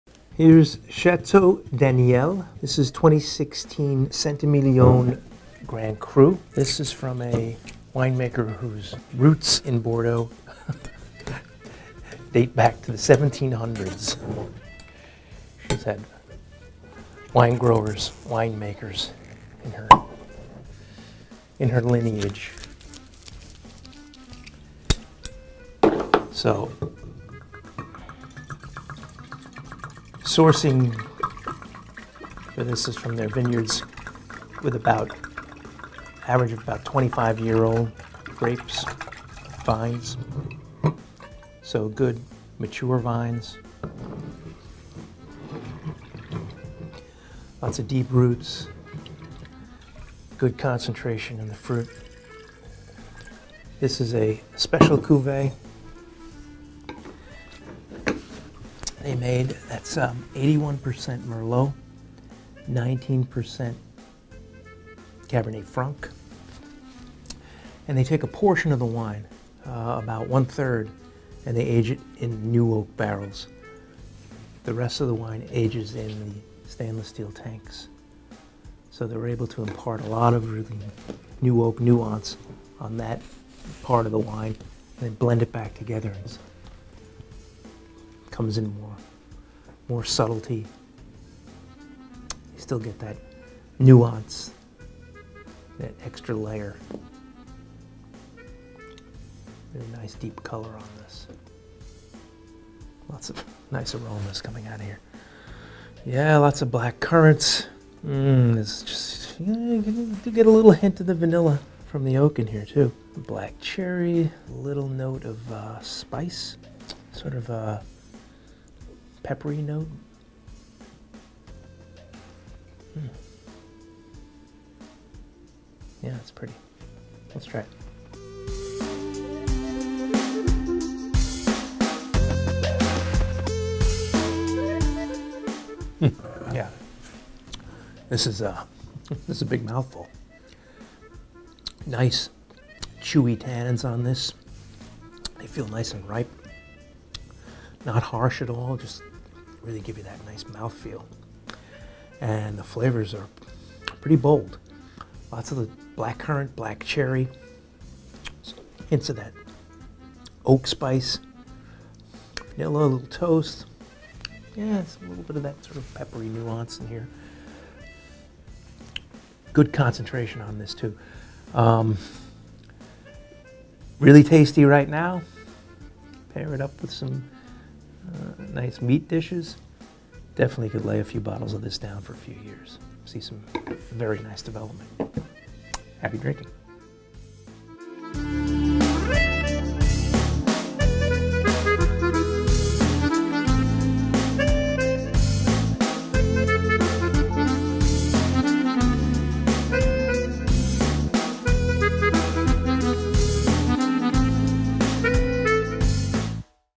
Chateau d'Anielle 2016 Saint-Emilion Grand Cru
Vanessa Albessard-Aubert is the descendant of a family of wine makers. She has grown up between the vineyards and cellars her ancestors have cultivated on the Right Bank of Bordeaux since 1732.